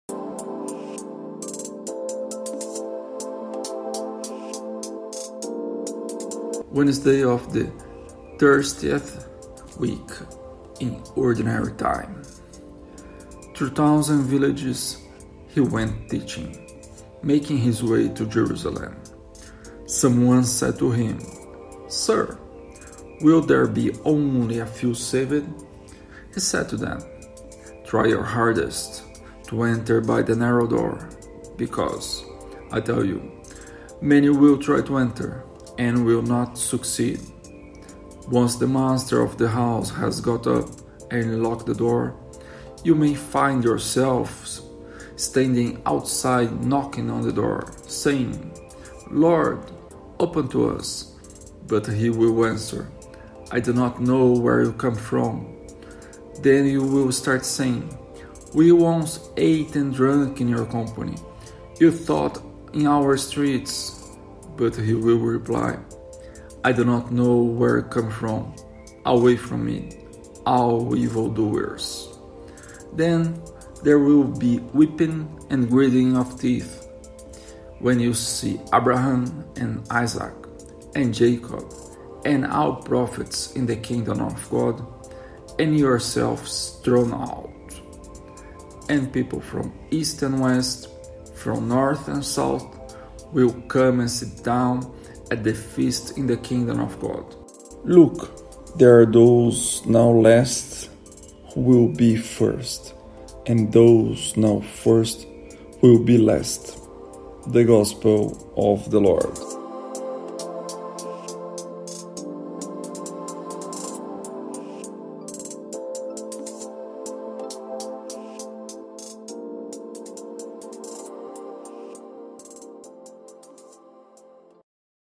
Holy Gospel 30 October 2019 Luke Ch 13 22 - 30 (made with Spreaker)
Wednesday of the Thirtieth Week in Ordinary Time

"22.Through towns and villages he went teaching, making his way to Jerusalem. 23.Someone said to him, 'Sir, will there be only a few saved?' He said to them, 24.'Try your hardest to enter by the narrow door, because, I tell you, many will try to enter and will not succeed. 25.'Once the master of the house has got up and locked the door, you may find yourself standing outside knocking on the door, saying, "Lord, open to us," but he will answer, "I do not know where you come from." 26.Then you will start saying, "We once ate and drank in your company; you taught in our streets," 27.but he will reply, "I do not know where you come from; away from me, all evil doers!" 28.'Then there will be weeping and grinding of teeth, when you see Abraham and Isaac and Jacob and all the prophets in the kingdom of God, and yourselves thrown out. 29.And people from east and west, from north and south, will come and sit down at the feast in the kingdom of God. 30.'Look, there are those now last who will be first, and those now first who will be last.'"